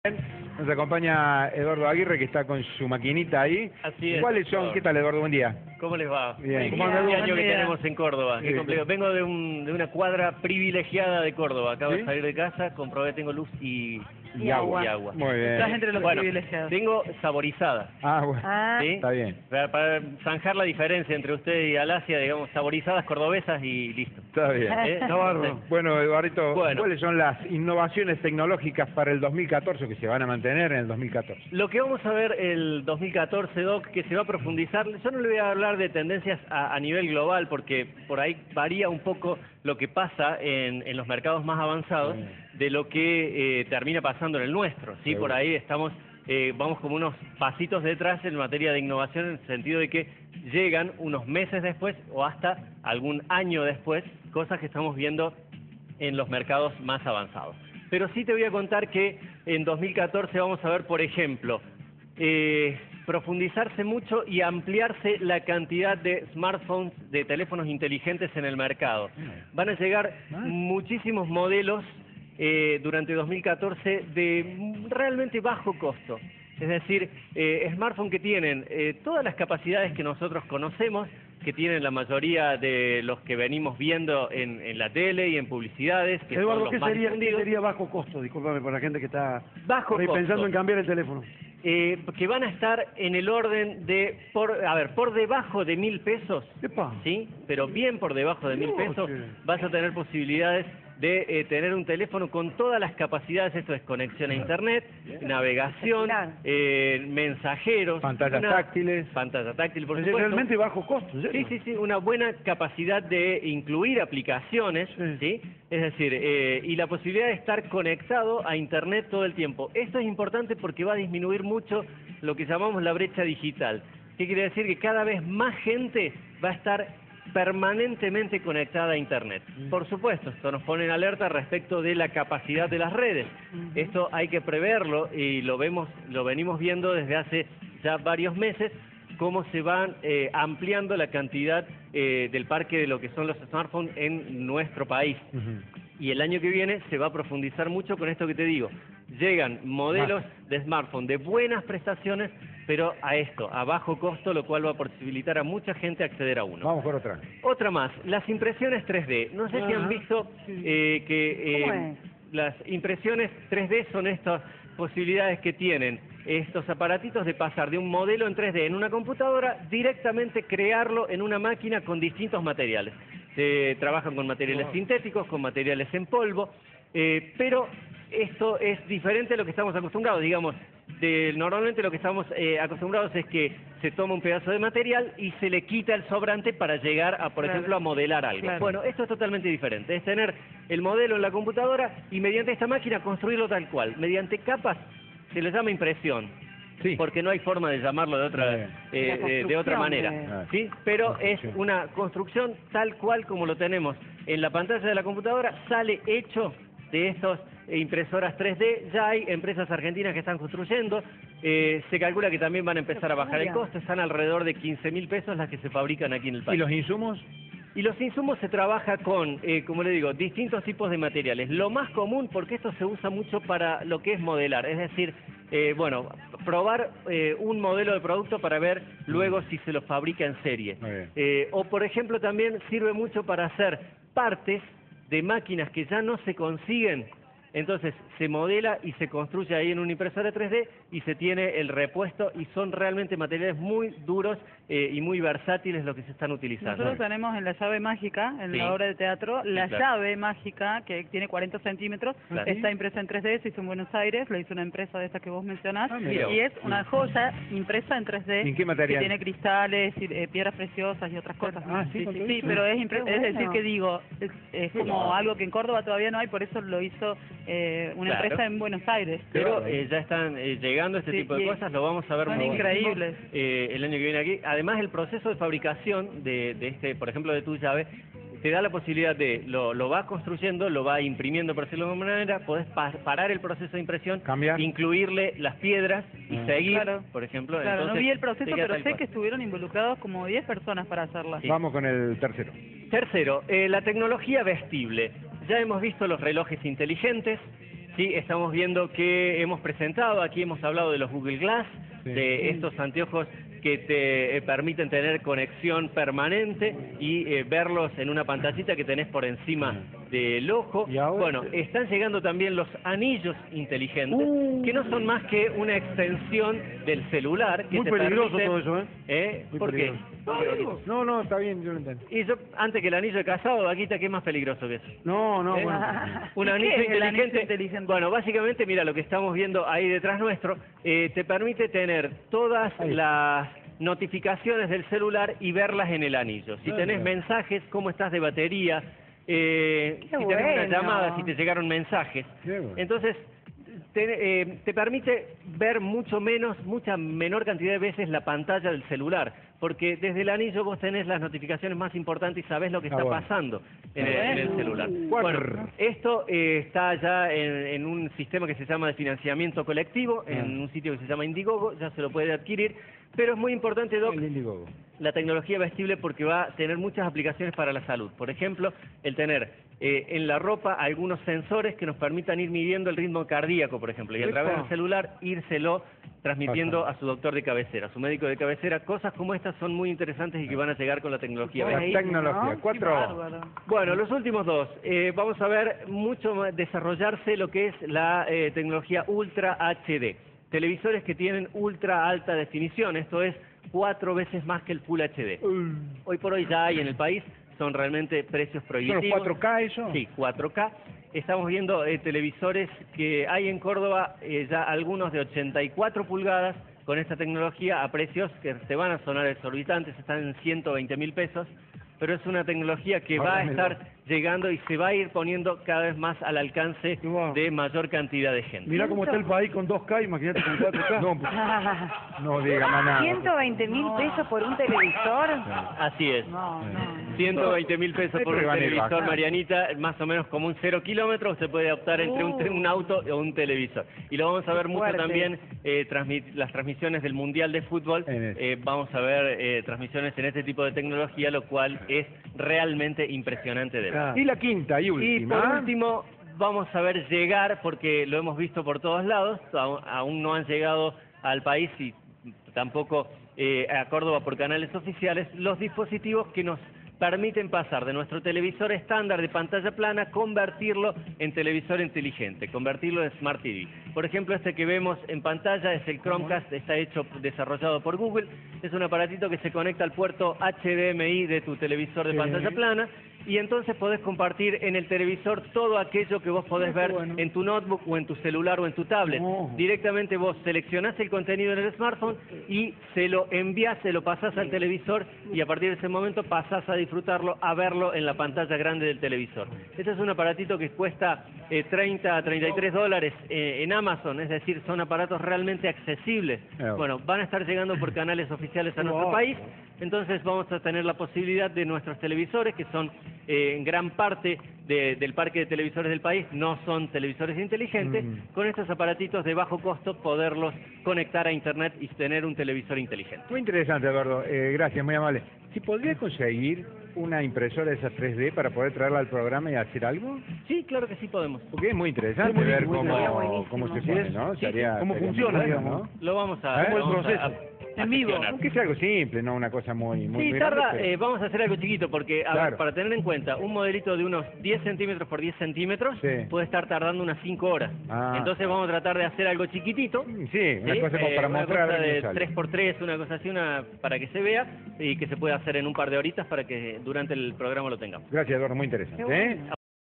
Cinco tendencias tecnológicas que prevalecerán 26 12 2013
Eduardo Aguirre, periodista especializado en tecnología, nos ofrece una guía completa de las novedades tecnológicas, así como las recomedaciones y los tips para aprovecharla al máximo. En esta ocasión, Eduardo ofrece un análisis de las cinco tendencias tecnológicas que prevalecerán en el 2014.